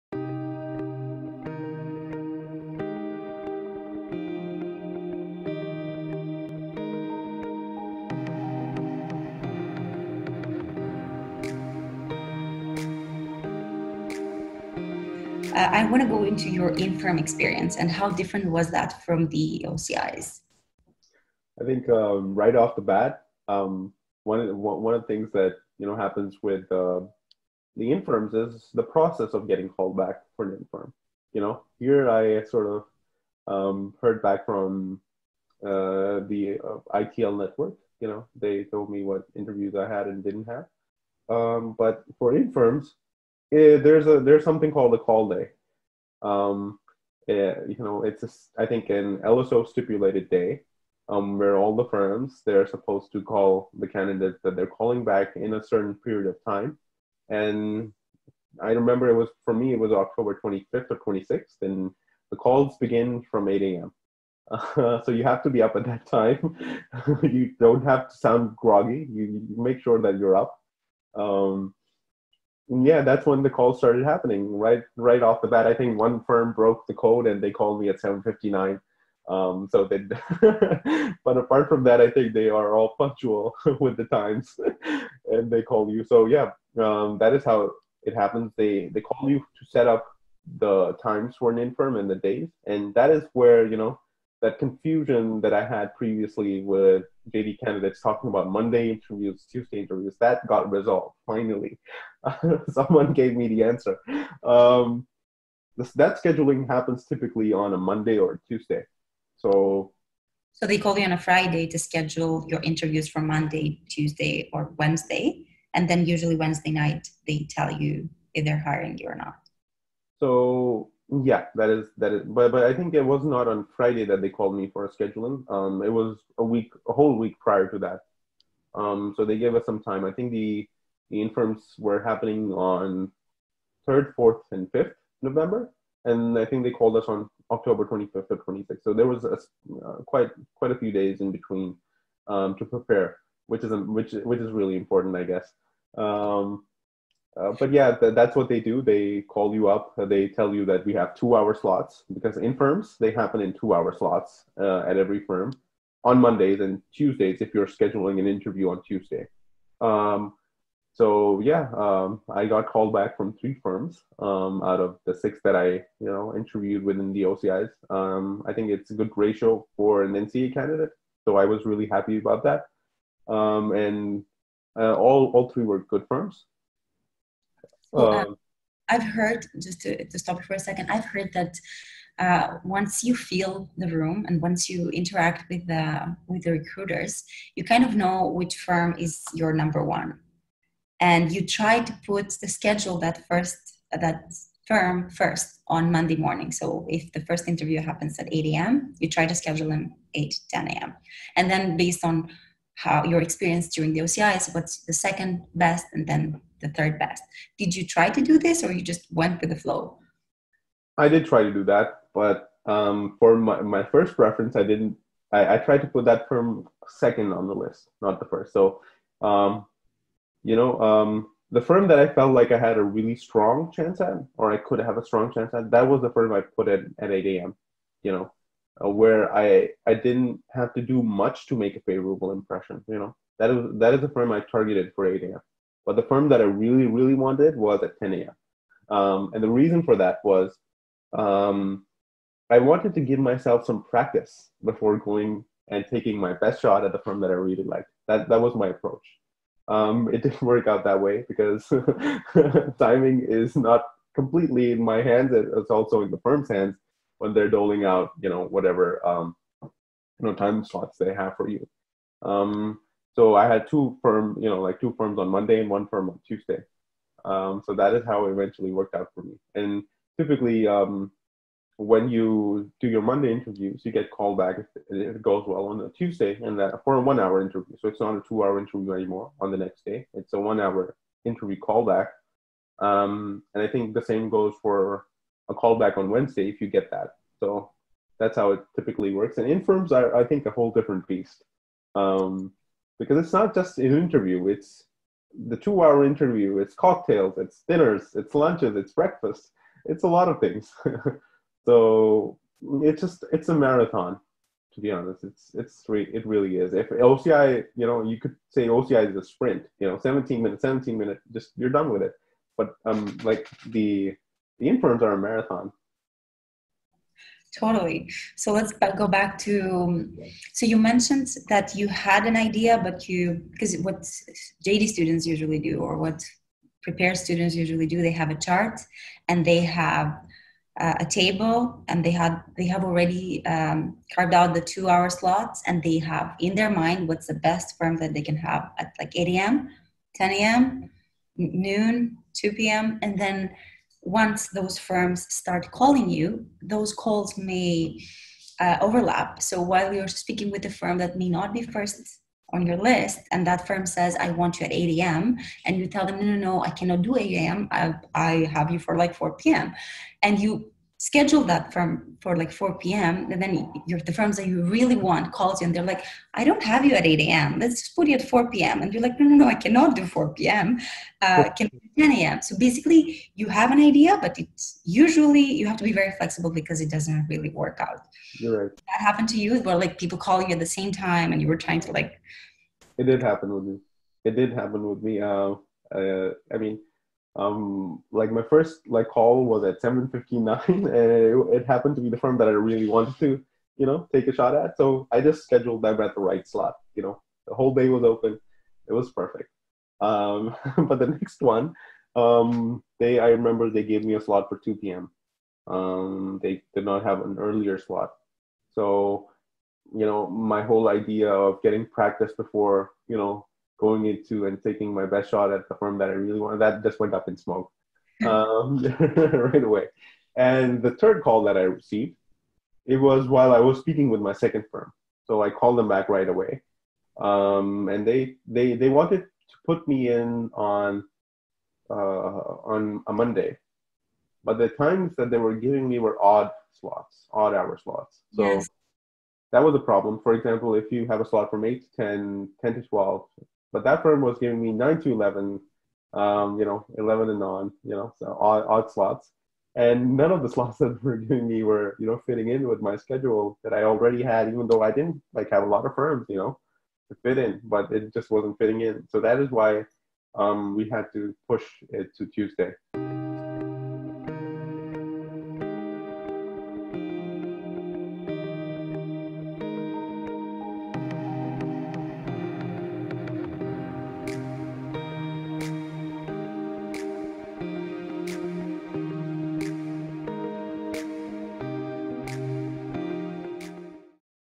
Interview Differences Between OCIs and In Firm; Scheduling
Yonida Koukio interviews Internationally Trained Lawyers and NCA Candidates (ITLNCAs) who participated in the On Campus Interviews (OCIs) for ITLNCAs, who secured positions through the process. Get these insights to design your strategies.

Here, Mohammad Awais Leghari answers: How different were the in firm interviews from the OCIs? And, How does the scheduling work?